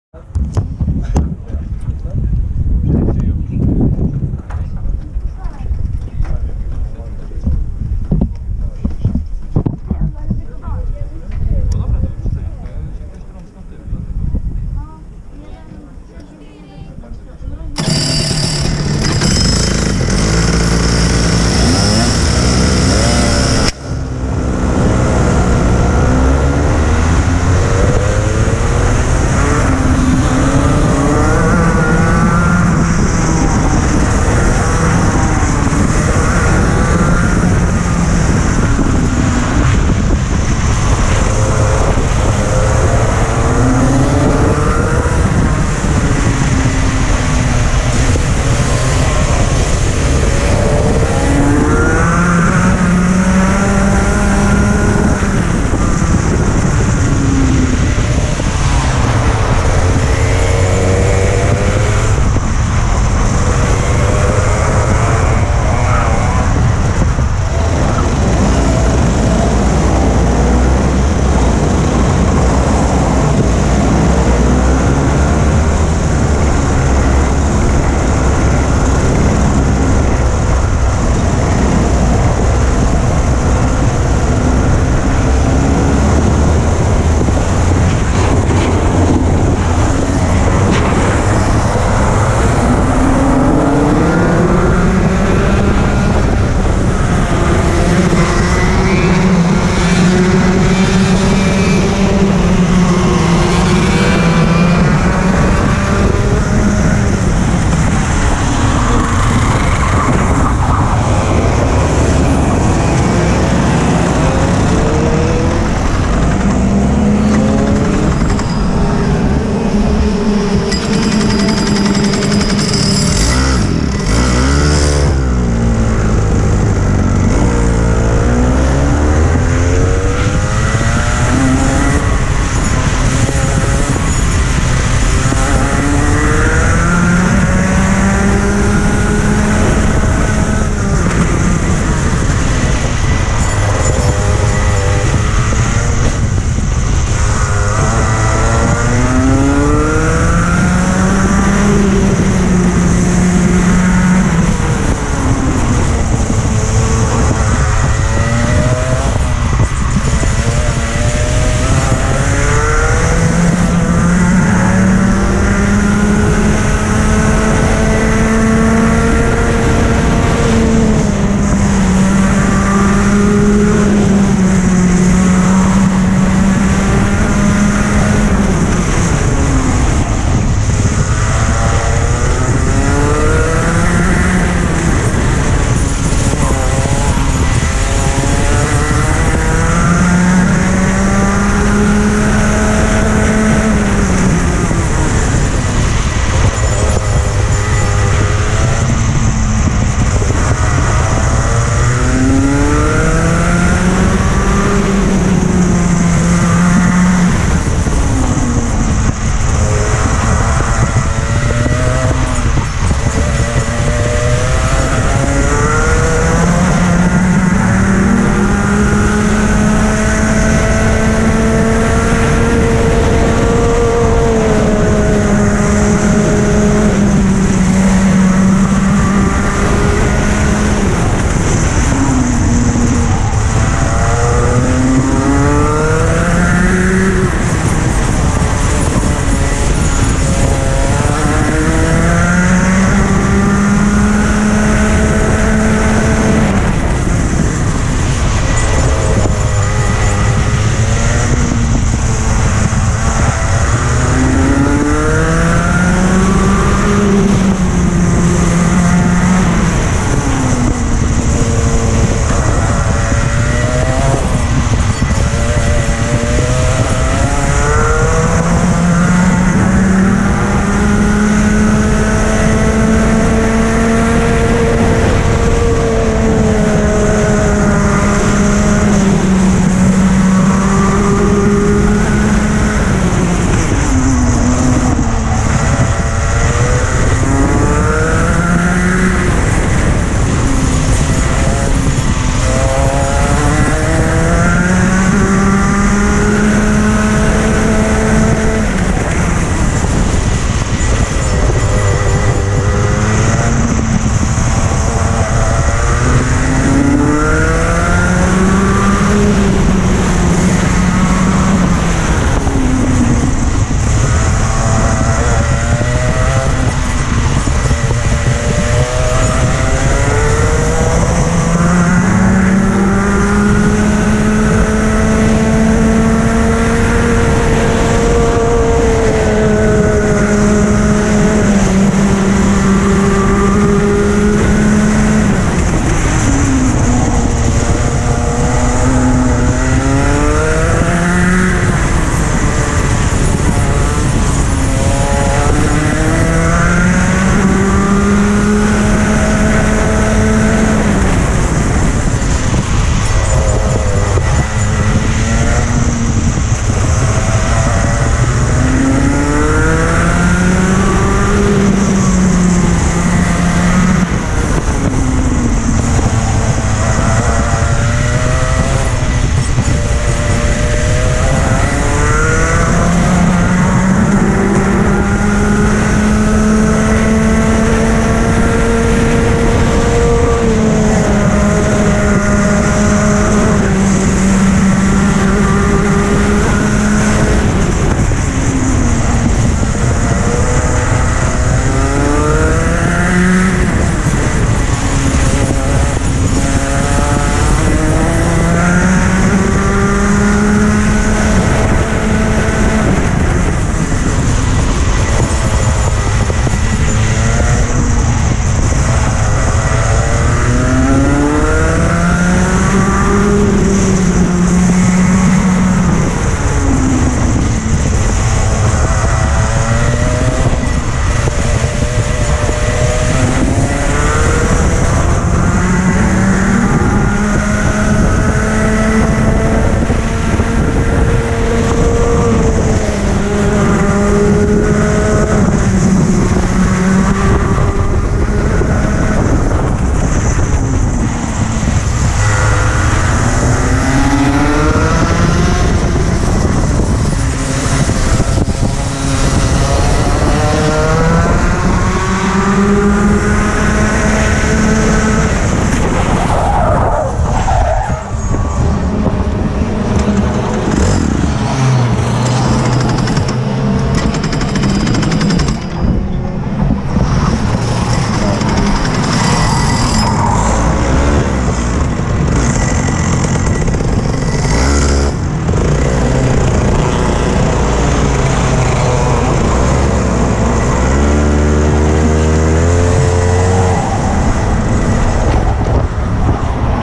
Felipe Nardo - Rotax Max 125 - Steel Ring - 10.2023 - Evolution Cup - Fast Layout - Heat 2
Started P9
DNF
Crash at 07:30
There were Rotax and OK karts on the same grid

Chassis: OTK Kosmic
Engine: Rotax Max 125cc - 2 strokes
Tyres: Mojo D5